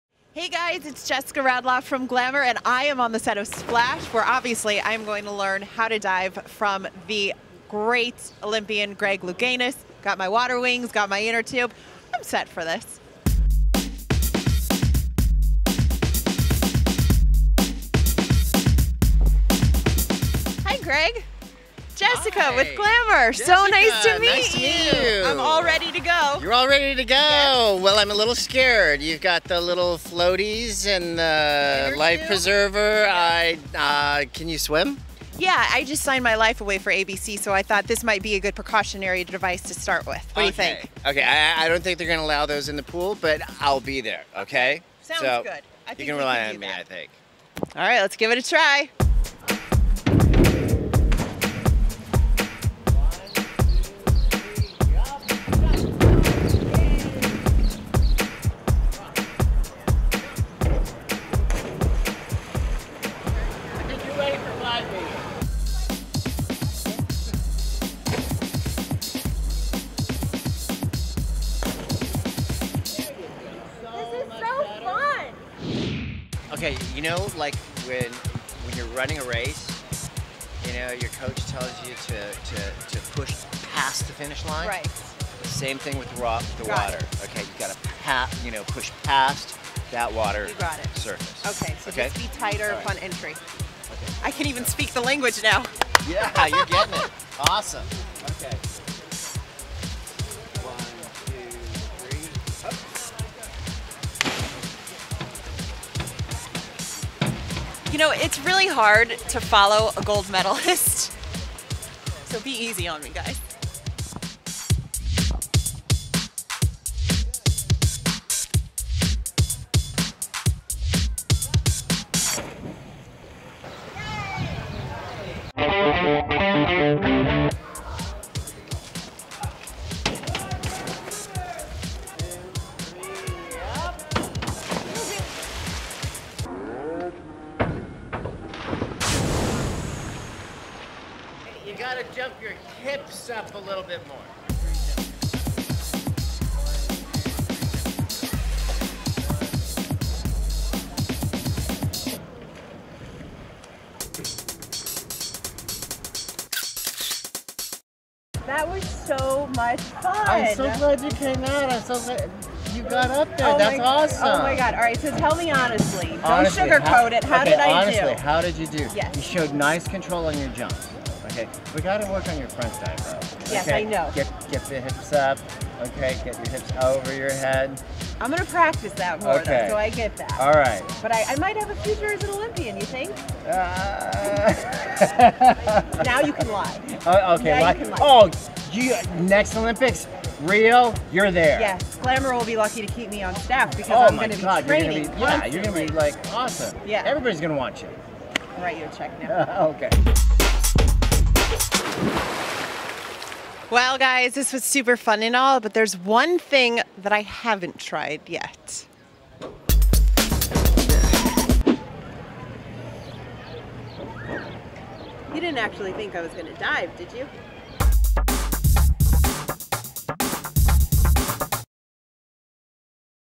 Splash's Greg Louganis Teaches Glamour How to Dive - Glamour
Splash star Greg Louganis shows our own Jessica Radloff what it takes to be a gold-medal winning diver—see what happens!

Your guide to the latest fashion trends, outfit ideas, hairstyles and makeup how-tos, and celebrity news.